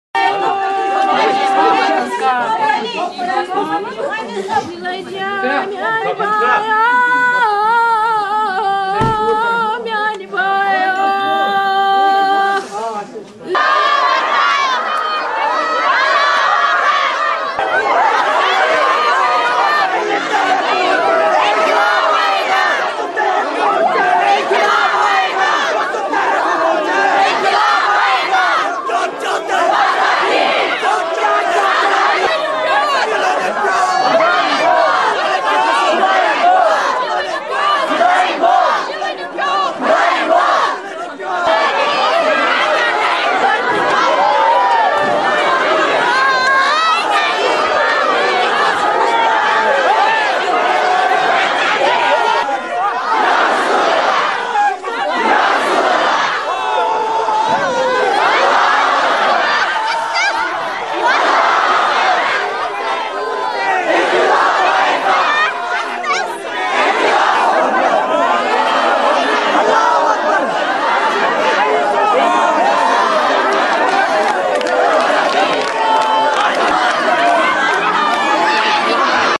People mourn Handwara Kashmir killings
People mourn ‪#‎Handwara‬ ‪#‎Kashmir‬ killings by Indian troops. Youth Iqbal Pir, Naeem Butt, Jehangir Ahmad Wani and Raja Begum killed many injured and massive clashes spread...